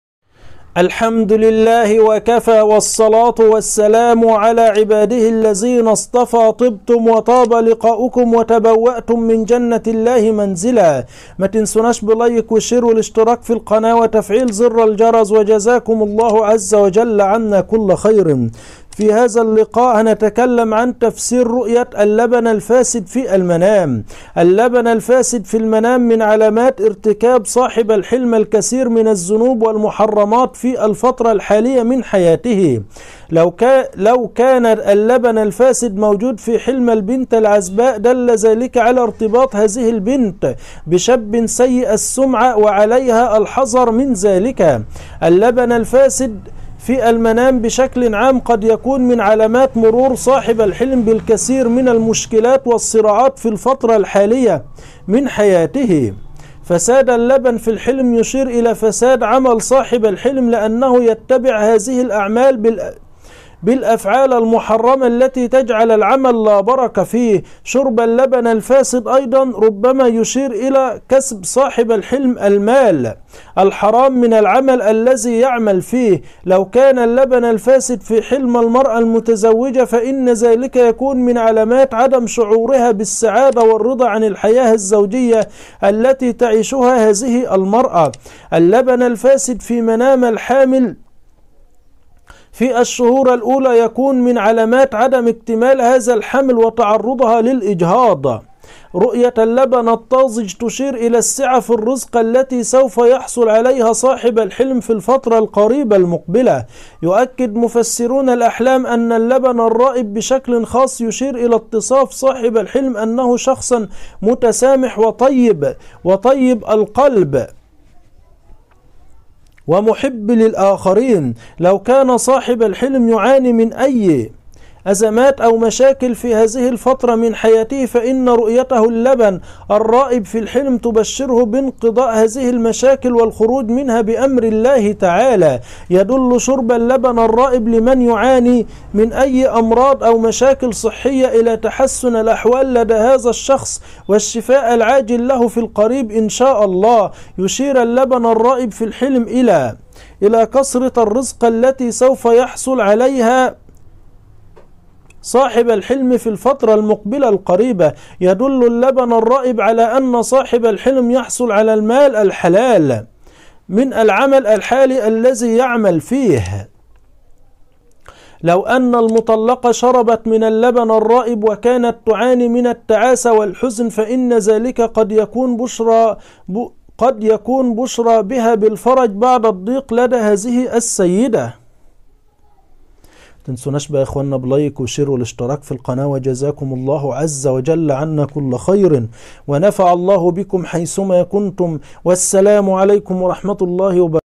تفسير حلم رؤية اللبن الفاسد في الحلم لابن سيرين والنابلسي
تفسير حلم رؤية اللبن الفاسد في المنام لابن سيرين والنابلسي بالتفصيل محمود أحمد منصور